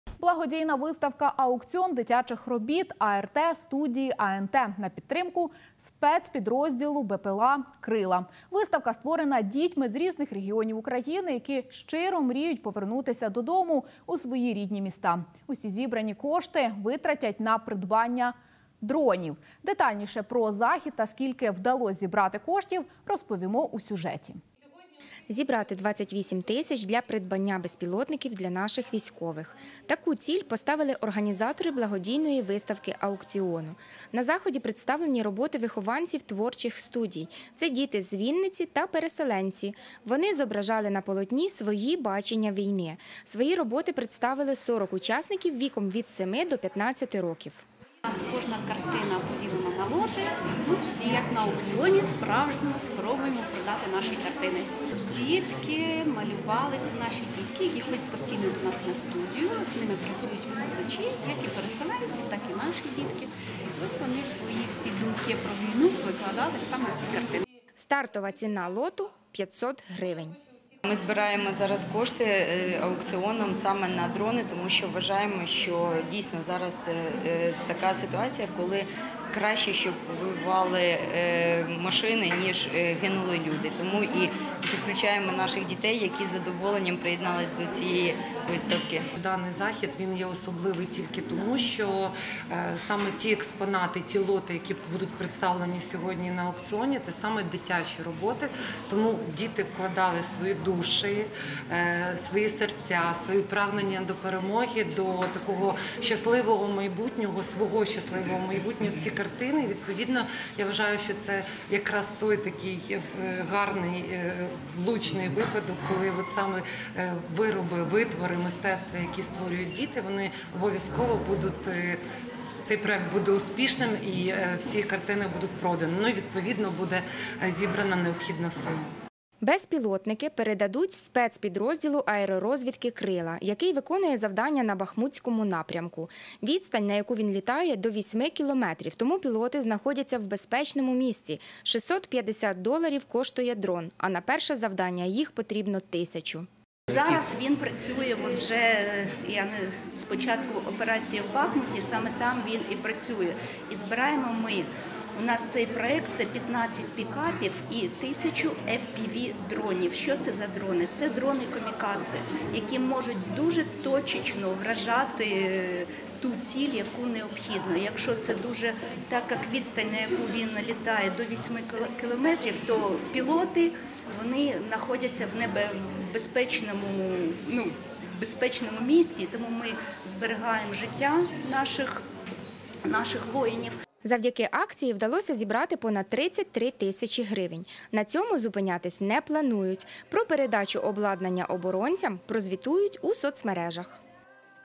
Благодійна виставка - аукціон дитячих робіт
Благодійна виставка - аукціон дитячих робіт арт студії "АНТ" на підтримку Спецпідрозділу БпЛА "КРИЛА". Виставка створена дітьми  з різних регіонів України, які щиро мріють повернутися до дому,  у свої рідні міста. Усі зібрані кошти витратять на придбання дронів.